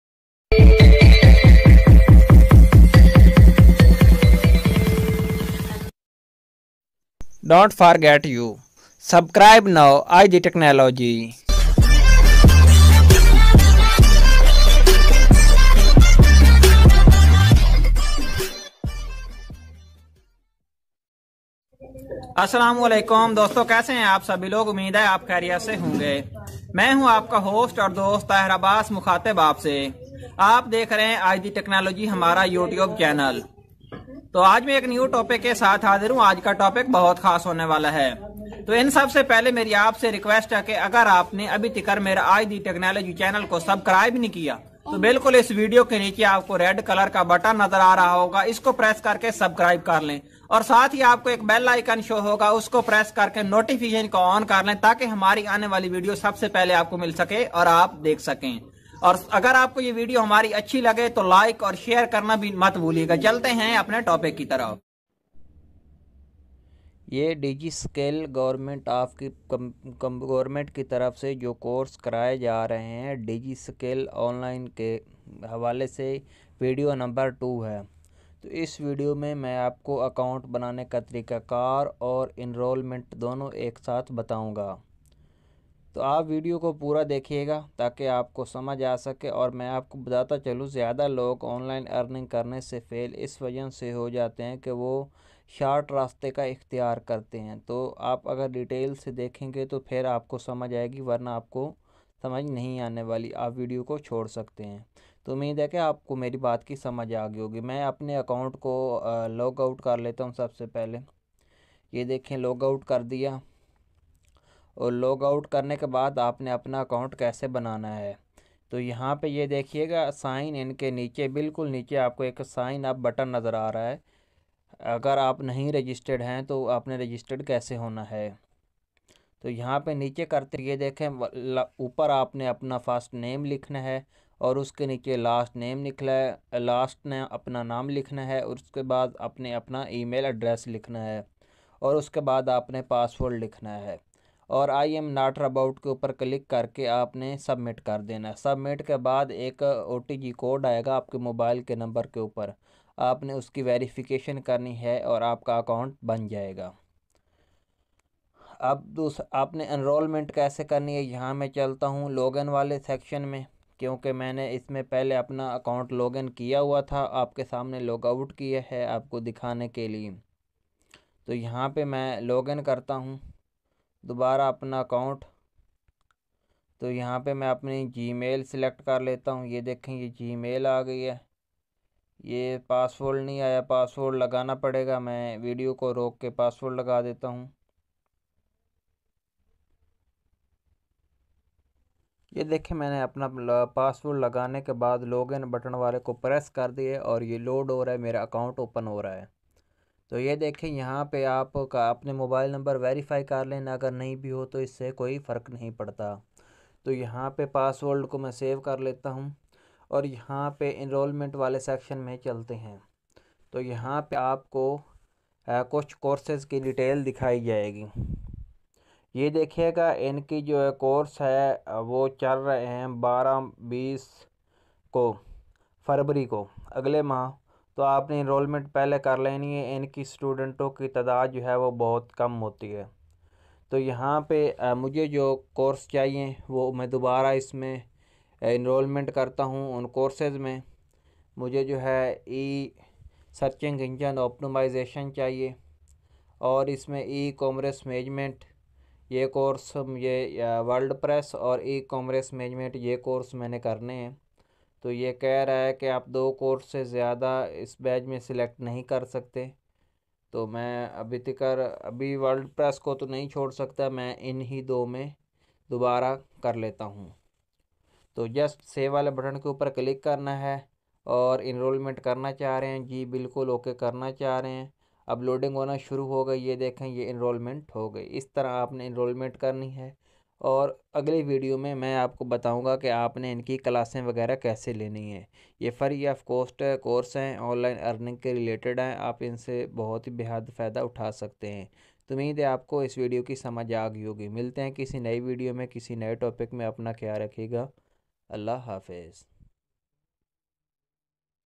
How to enroll course in digiskill LMS By Government of Pakistan
This video will show you that

Digiskills in online free plateform for all ages of people who want to learn anything new skill and after that you will be awarded with certificates that you can add in your cv and you will be also able to earn money online.
In this video I will help you to enroll in digiskill. I will show you with little easy steps to apply course and register in digiskills. Infact, will also show you how to use lms review from intro to last. After registeration you will be able to learn.

Digiskills program Pakistan Zindabad

About : Ajdi Technology is a YouTube Channel, where you will find technological videos in Urdu and  Hindi, New Video is Posted Everyday.,